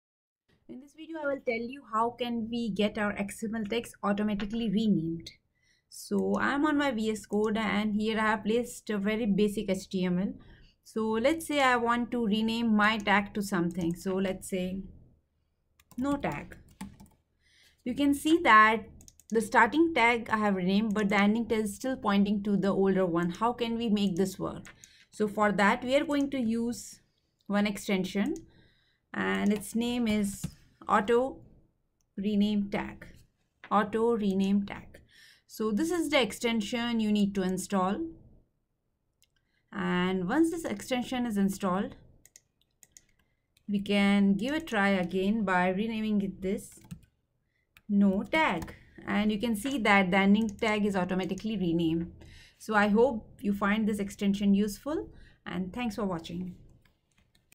Automatically Renaming XML Tags In Visual Studio Code
This video explains about how can we automatically rename the ending XML tag, whenever  starting tag is renamed.